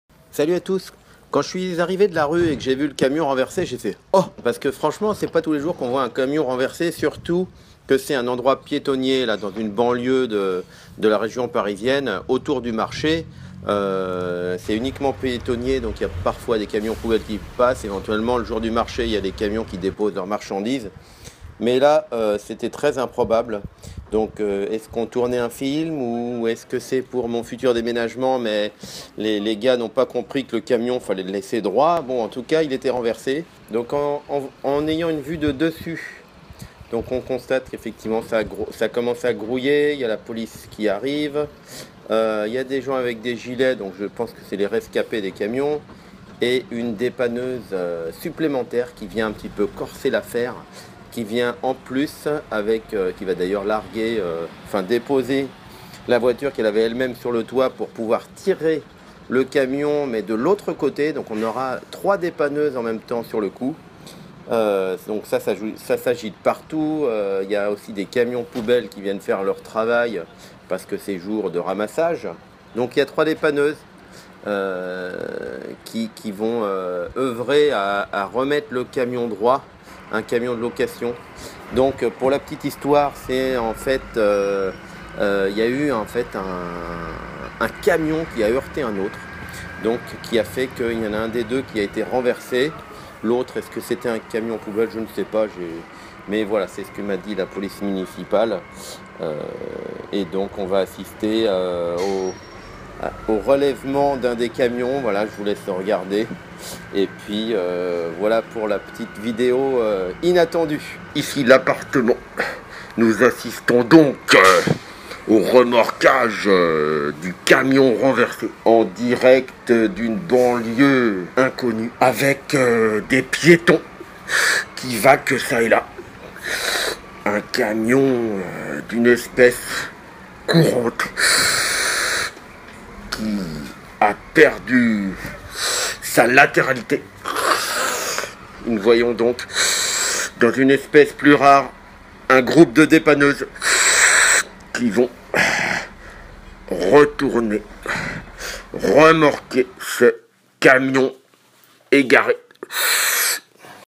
CAMION RENVERSE par le vent ! devant chez moi - dans une rue piétonne !
Vidéo incroyable d'un CAMION RENVERSE DEVANT CHEZ MOI ! 100% improbable en rue piétonne ! CAMION RENVERSE par le vent !